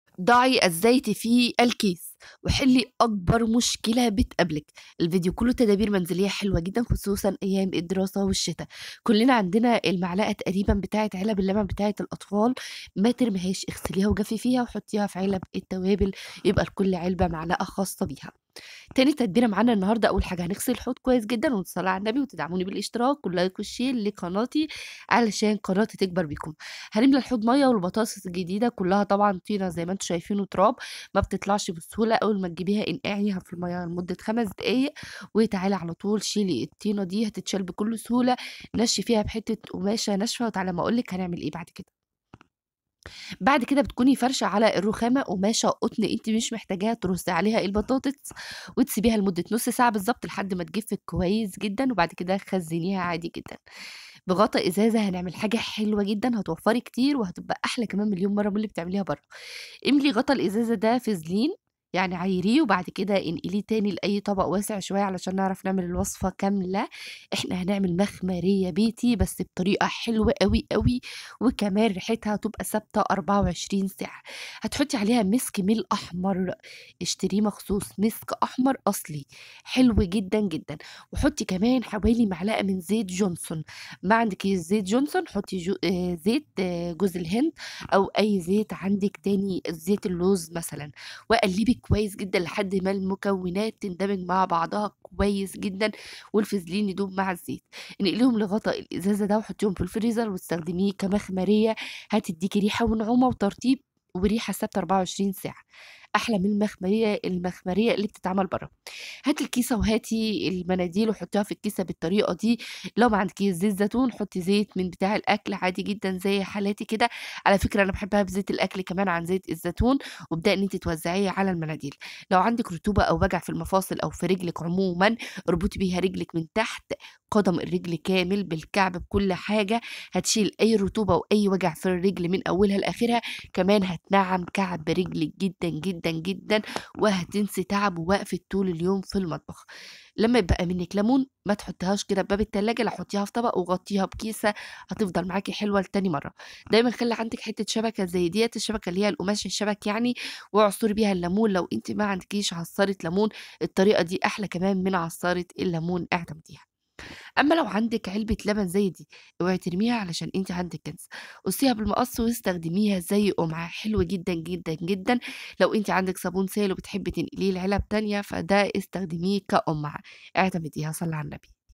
ضعي الزيت في الكيس وحلي اكبر مشكله بتقابلك💯 تدابير_منزلية هتريحك في الشتاء ب 5 حلول لمشاكل المطبخ✅
ضعي الزيت في الكيس وحلي اكبر مشكله بتقابلك💯 تدابير_منزلية هتريحك في الشتاء بخمس 5 حلول لمشاكل المطبخ✅